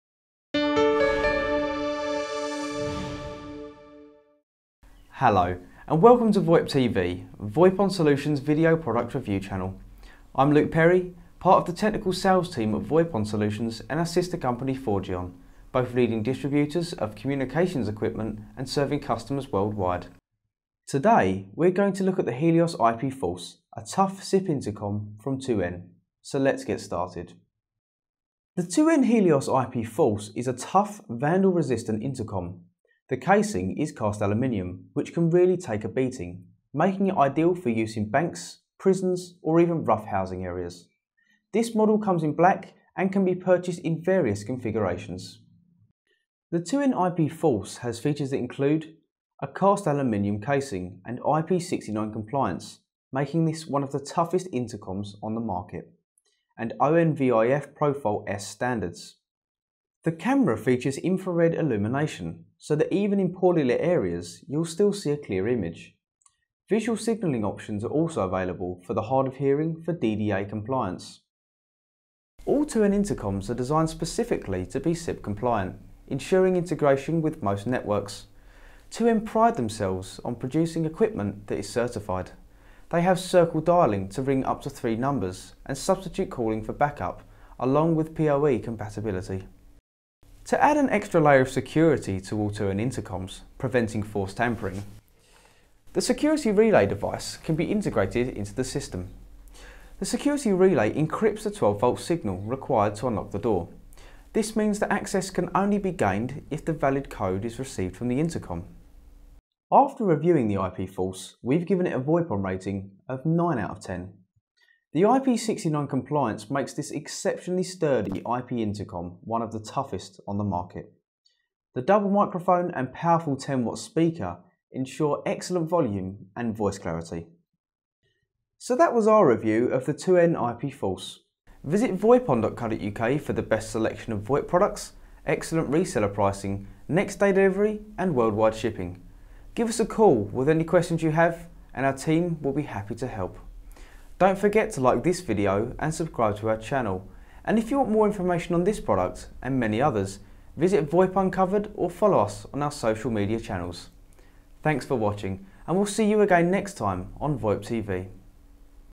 2N IP Helios Force Intercom System Video Review / Unboxing
Luke Perry, part of the Technical Sales team at VoIPon Solutions takes a look at the 2N Helios IP Force.

The 2N Helios IP Force is an exceptionally sturdy IP intercom that will ensure you effortless communication with the people coming to your door, in combination with reliable security for your building entrance. It supports voice and video communication using the SIP protocol and can deal with the most demanding conditions.